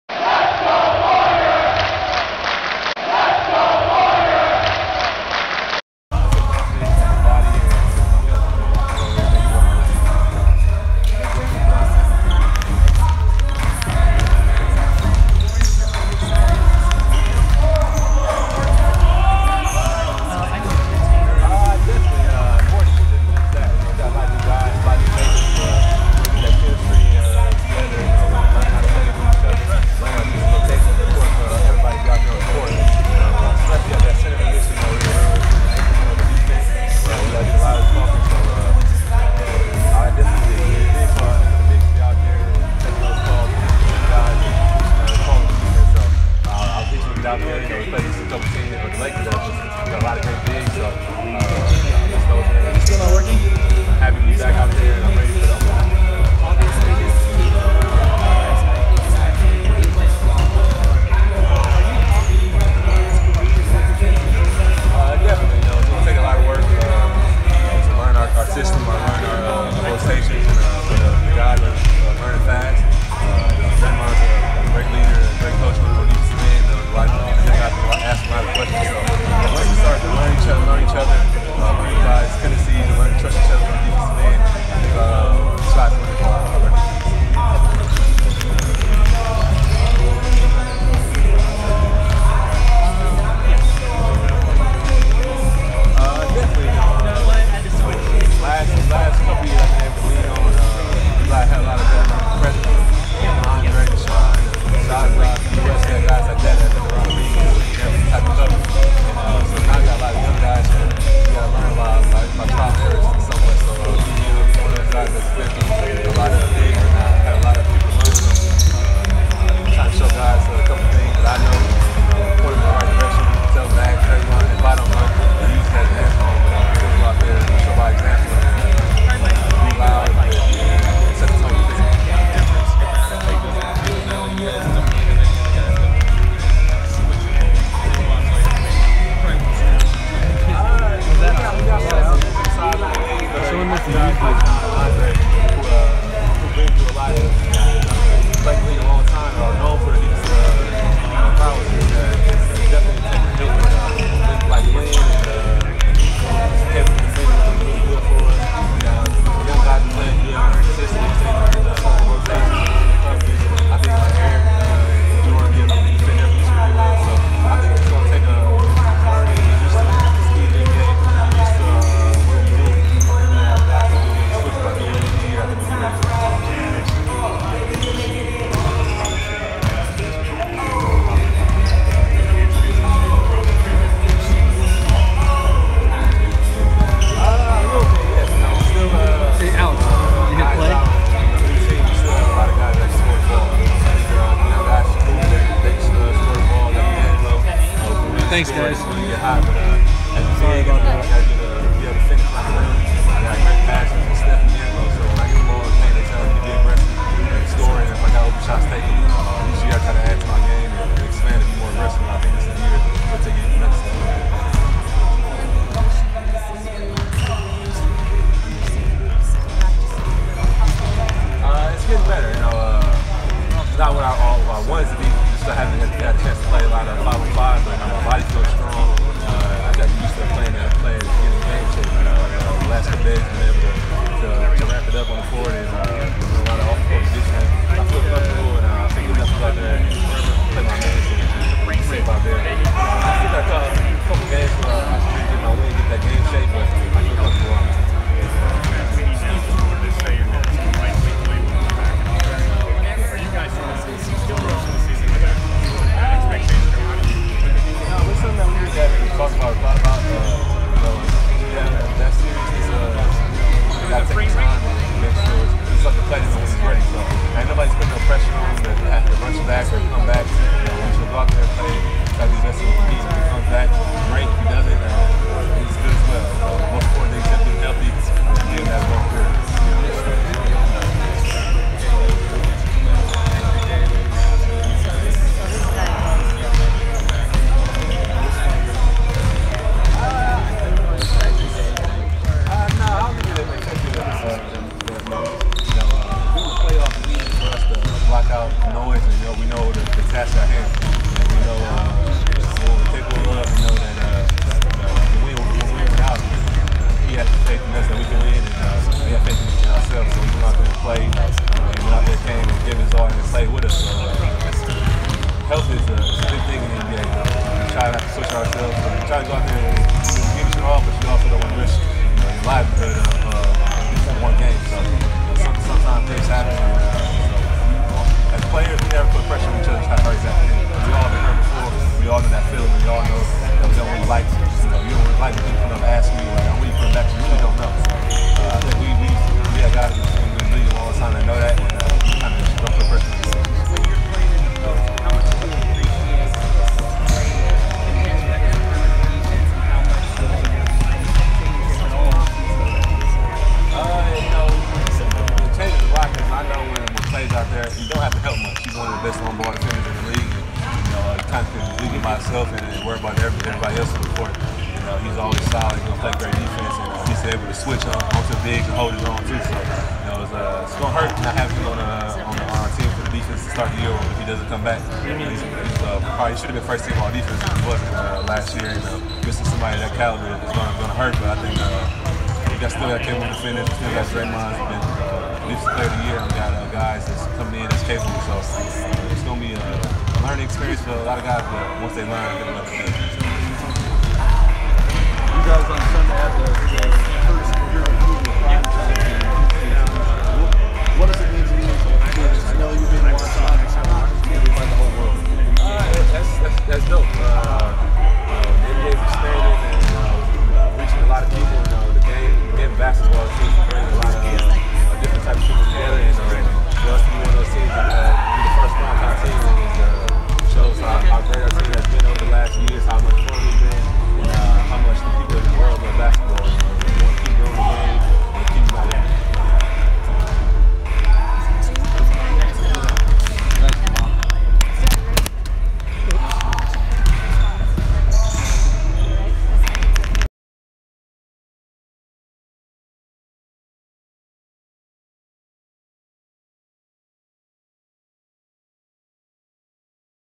[HD] Full LOONEY Q&A: “on track” and “95%” for Opening Night; on Klay’s return (or not)
From Golden State Warriors (0-0) practice, 4 days before season opener vs LA Clippers, at Biofreeze Performance Center inside Chase Center in San Francisco, CA.

PODCAST DETAILS...
All platforms (same link as above)